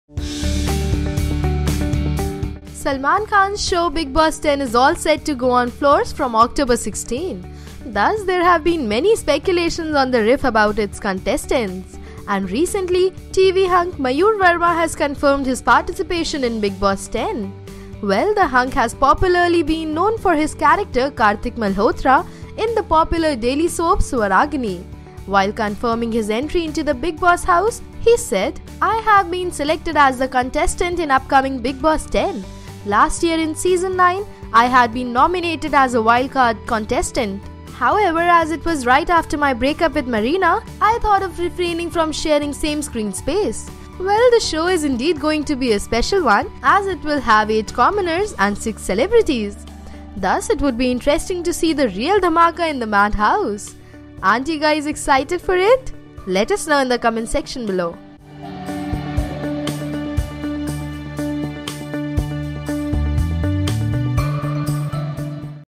Bigg Boss 10: TV Actor Mayur Verma To Enter Salman's Show
TV hunk Mayur Verma has been confirmed for Salman Khan's Bigg Boss 10. Check it out…